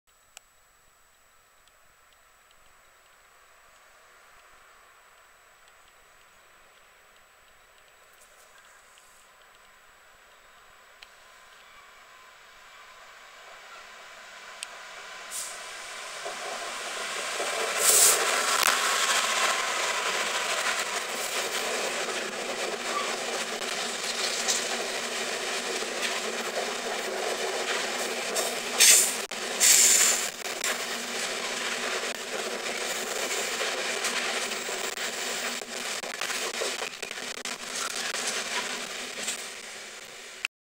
66187 6L77 08:27 Mountsorrel Sdgs to Marks Tey Tarmac @ Stansted Mountfitchet 03/12/13
Class 66 diesel locomotive 66187 in EWS livery is seen passing through Stansted Mountfitchet @ 12:07 running 2 minutes early hauling Bardon type stone wagons and working 6L77 08:27 Mountsorrel Sdgs to Marks Tey Tarmac